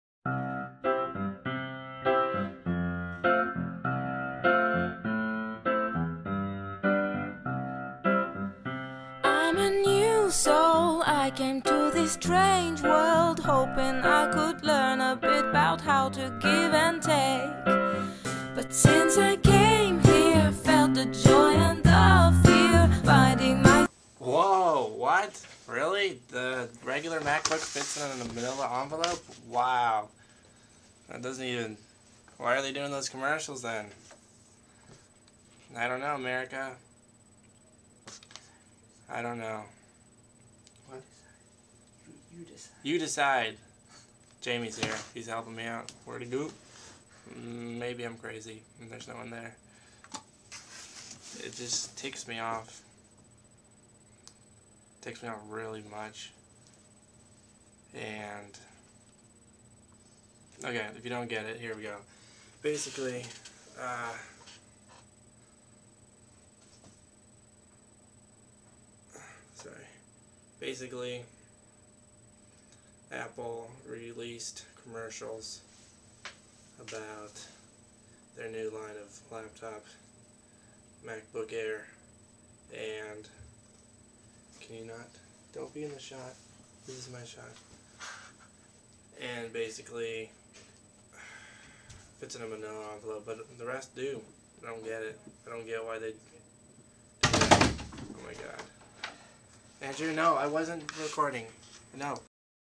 Macbook AIR is a fraud!!!!
Macbook AIR &  Steve Jobs have a campaign about nothing new!  Your old laptops fit in an envelope!  UGH!  Macbook Air is still tight.  Just want a optical drive.